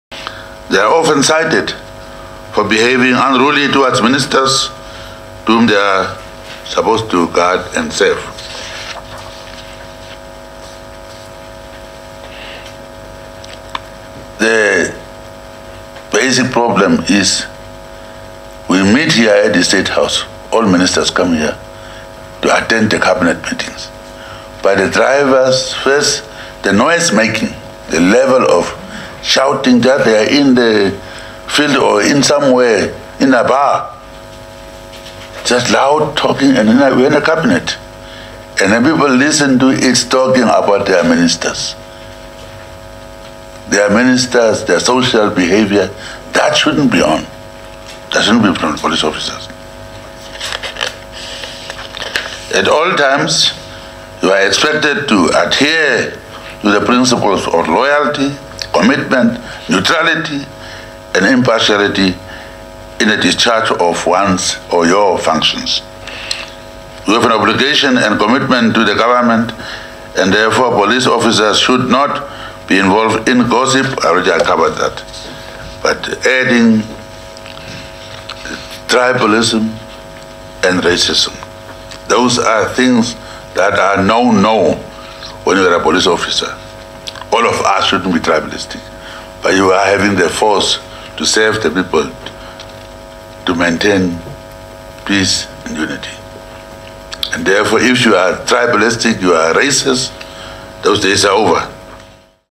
NAMPA WHK Geingob calls VVIP drivers to order (12 DEC 2018)
WINDHOEK, 12 DEC (NAMPA) – President Hage Geingob has raised disappointment over the ill behaviour of some Namibian Police Force (NamPol) Very Very Important Person designated drivers. Geingob said this in a meeting with NamPol Inspector-General, Lieutenant-General Sebastian Ndeitunga and his high ranking subordinates at State House on Wednesday. (Photo by: George Hendricks) NAMPA.